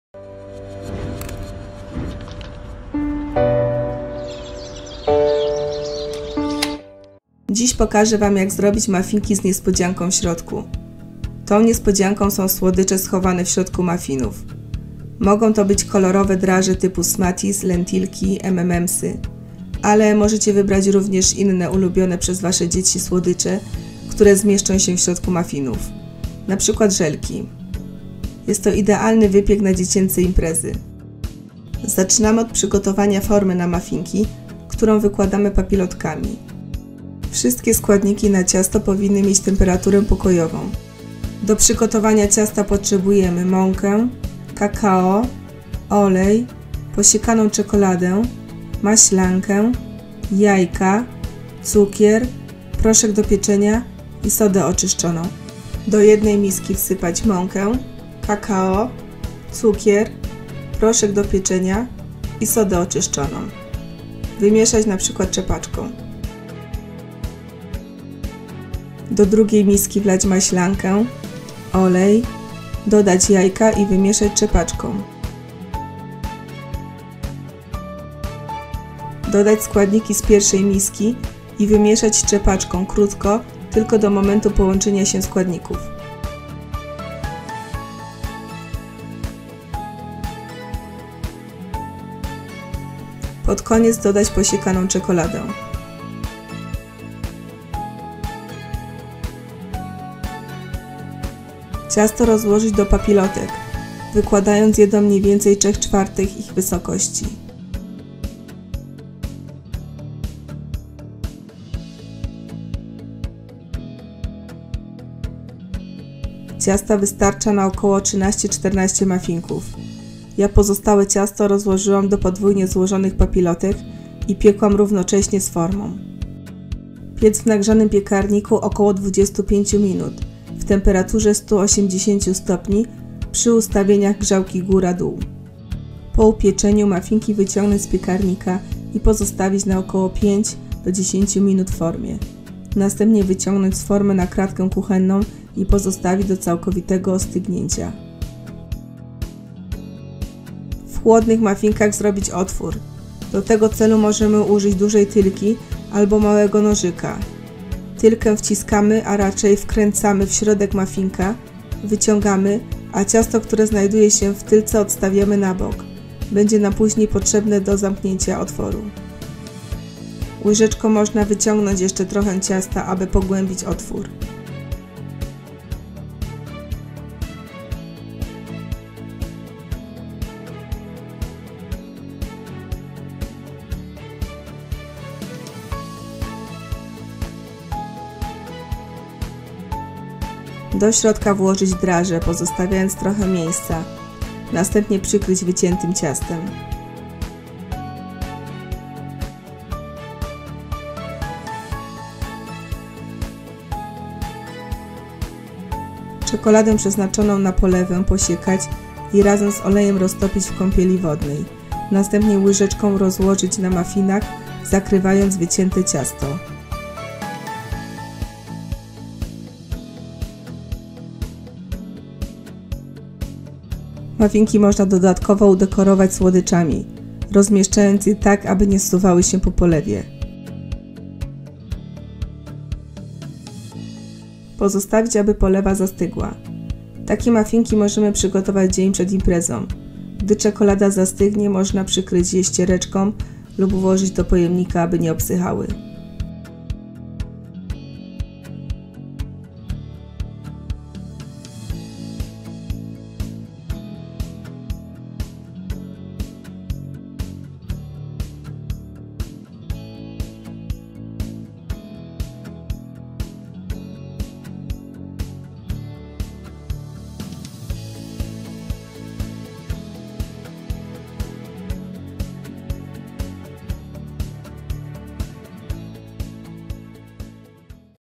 Muffinki z niespodzianką ~ Domowe Wypieki ~
Czekoladowe muffinki z niespodzianką w środku. Są szybkie i łatwe do przygotowania. Sprawdzą się z pewnością na dziecięcych imprezach. Nie dość, że są czekoladowe i udekorowane kolorowymi drażami to jeszcze w środku czeka niespodzianka.